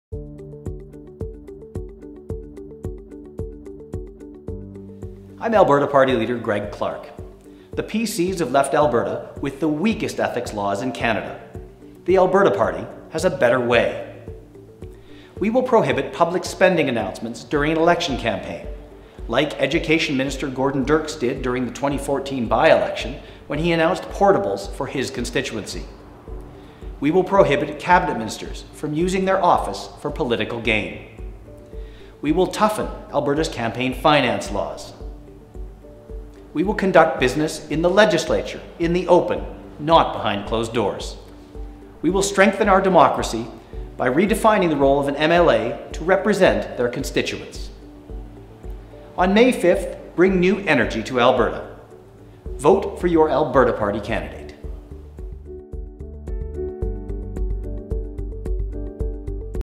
Alberta Party Ethics Platform
Listen to the Alberta Party's views on instilling a stronger ethics policy in Alberta politics. The full party platform can be found